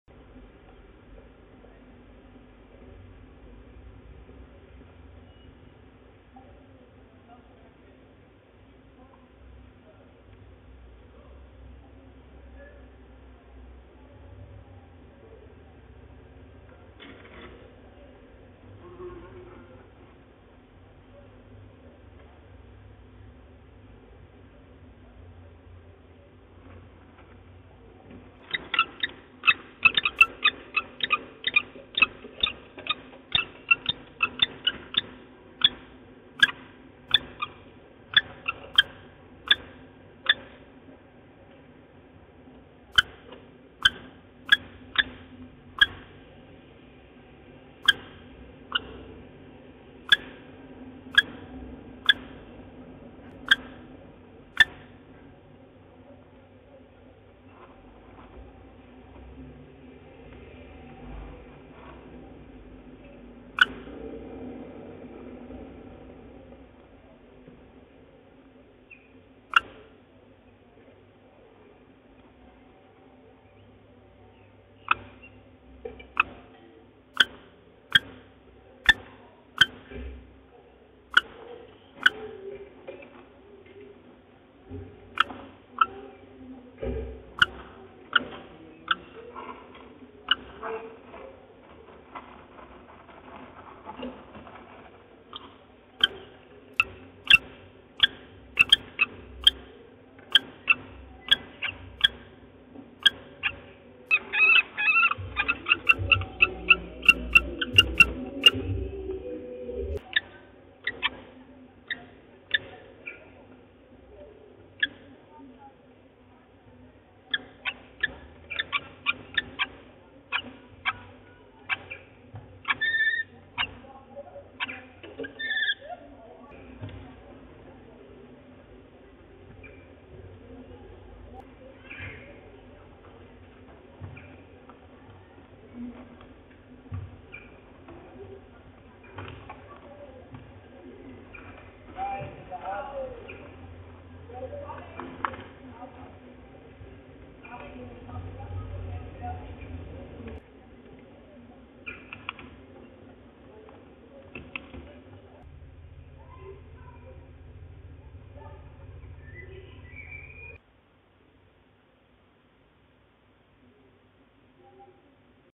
Pustułki - Sissi i Franz - 11 04 2024
Sowy już nie przychodzą i Sissi ma spokój. Franz przynosi jej
przysmaki i razem zaczynają spokojną inkubację pięciu jajek.
Nagrane z kanału - Große Kreisstadt Weißenburg i. Bay.